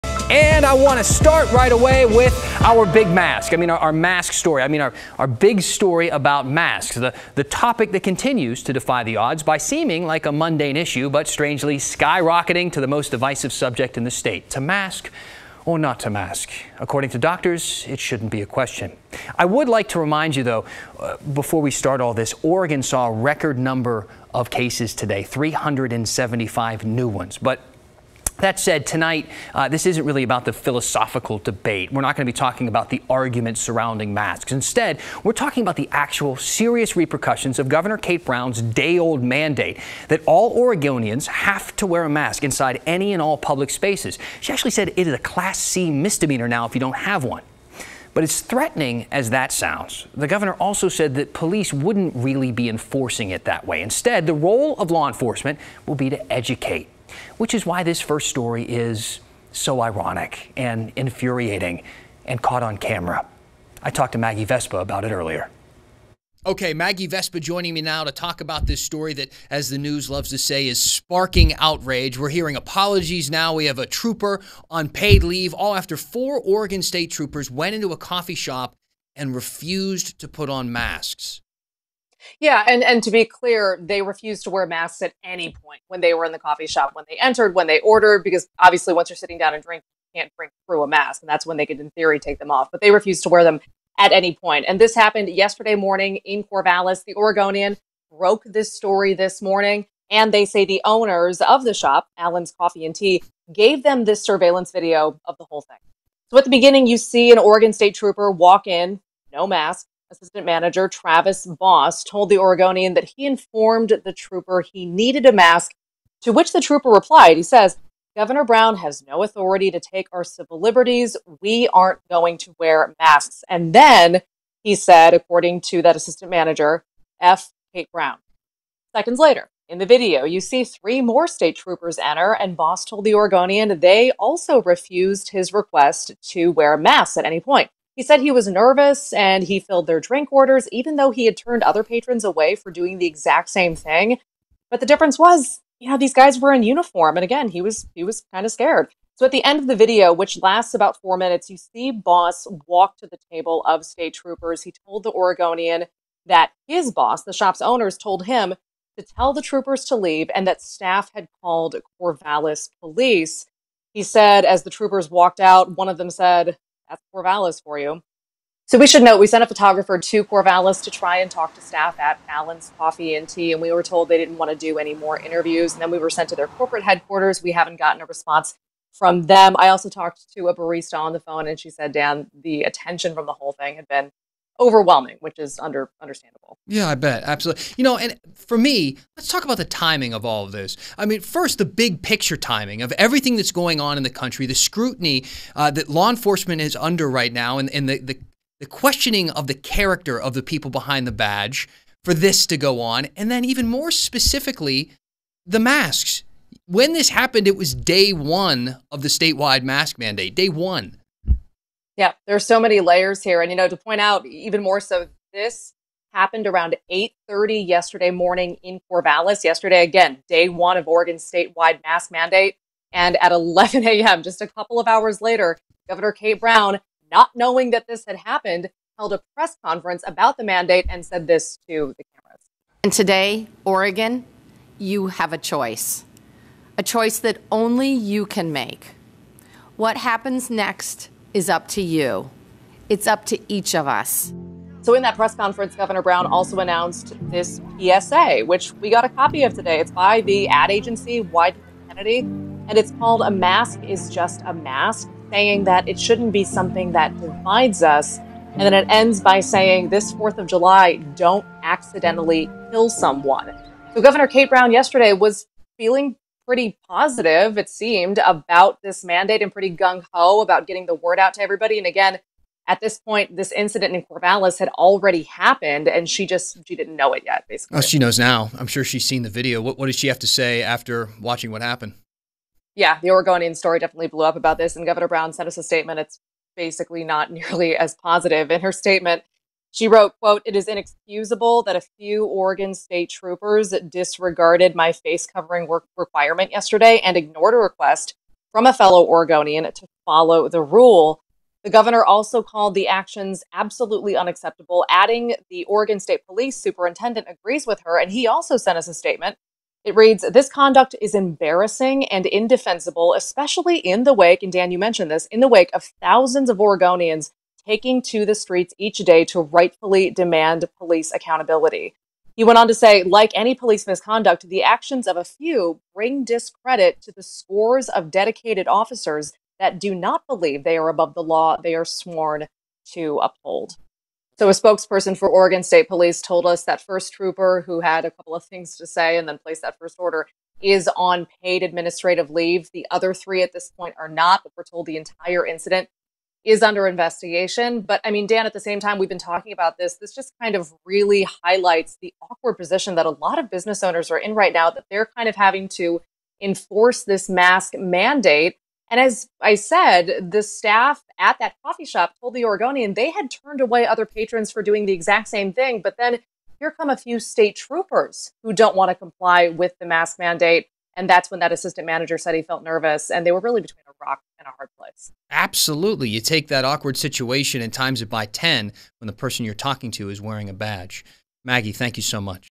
Troopers refuse to wear face masks in Oregon coffee shop
Despite a statewide requirement to wear face masks in public indoor places, OSP troopers were caught on camera refusing to do so.